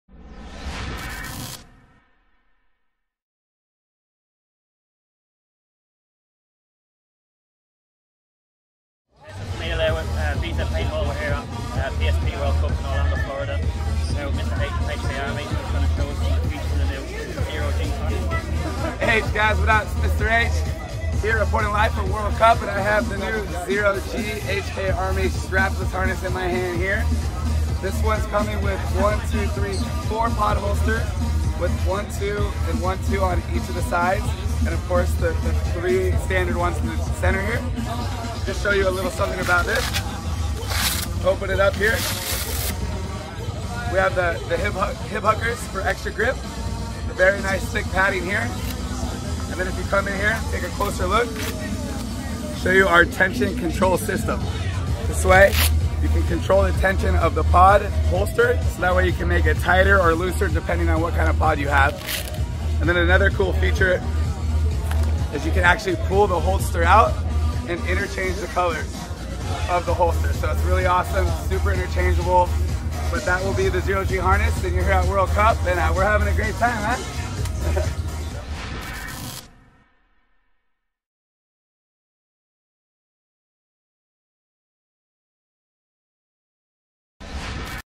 HK Army ZerO-G Harness Overview by BZ Paintball & Mr.H
An Overview of HK Army ZerO-G Harness Features by BZ Paintball & Mr.H from PSP World Cup 2014.

Available to order soon from bzpaintball.co.uk